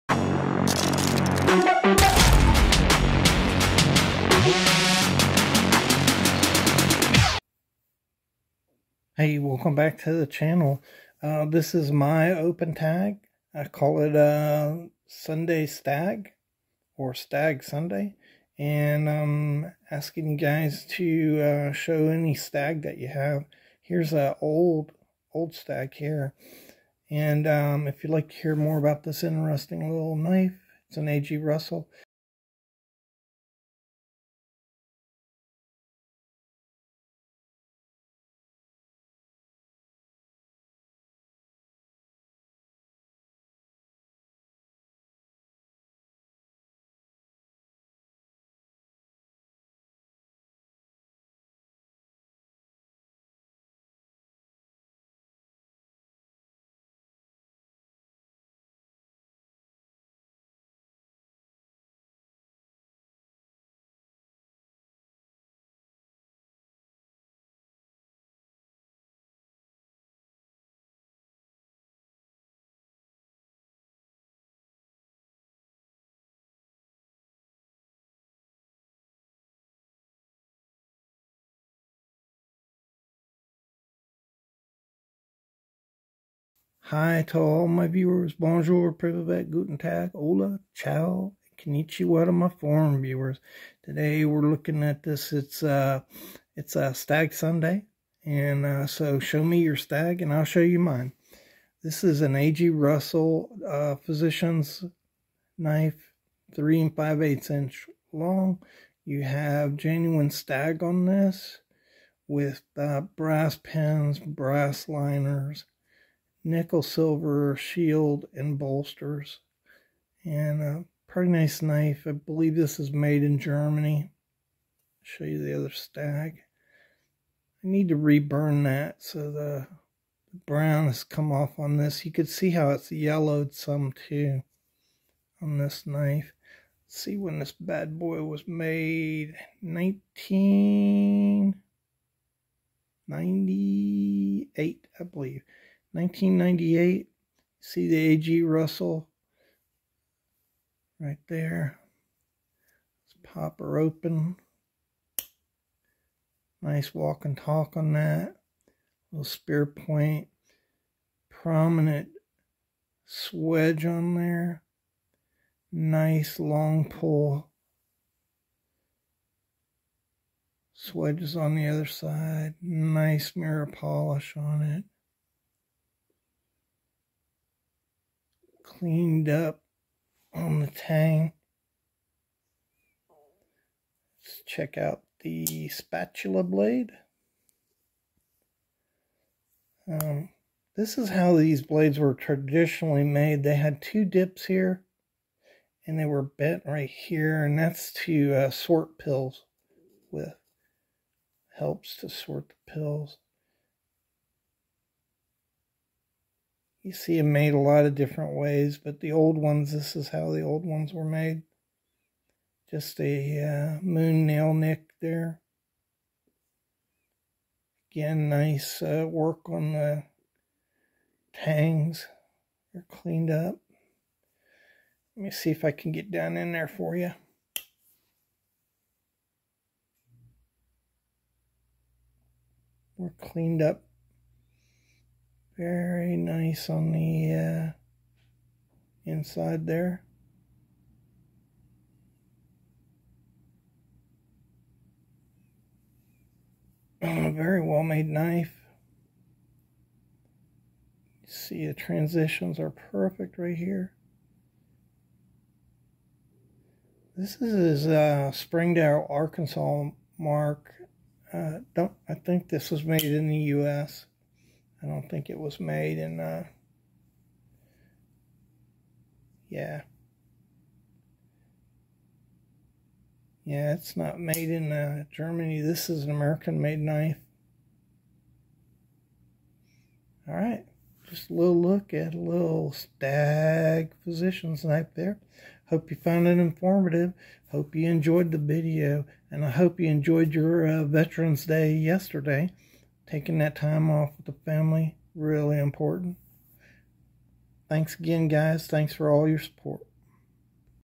A.G. Russell Physicians knife.
Another edition of Stag Sunday, show me your stag! An A.G. Russell Physicians knife from 1998.